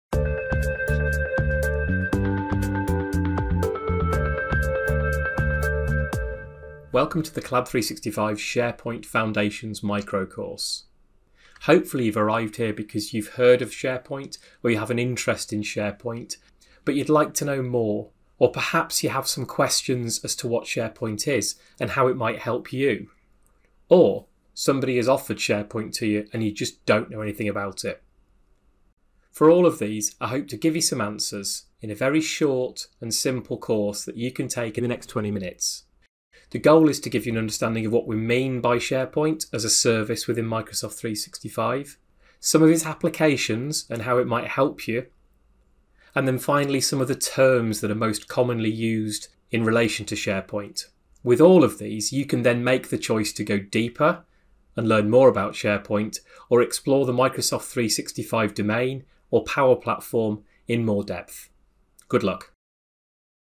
Welcome To The FREE SharePoint Online Foundations Course
Embark on your SharePoint journey with our free, beginner-friendly course, designed to teach you the essentials in under 20 minutes.

This course is the perfect starting point for those new to SharePoint Online, providing a clear and concise introduction to its core features. Learn what SharePoint is, how it functions, and the various ways it can be utilized in a business context. We'll guide you through the basics, including the different site types and templates, and how to use web parts effectively.

As the course progresses, delve deeper into creating and managing lists and document libraries, navigating site settings, and understanding the key differences between classic and modern views. Additionally, get a sneak peek into how SharePoint integrates with the Power Platform, enhancing its capabilities. This course is not just about theoretical knowledge; it’s about giving you the practical tools to start using SharePoint confidently in your daily work. Ideal for beginners, it simplifies complex concepts into digestible, easy-to-apply lessons.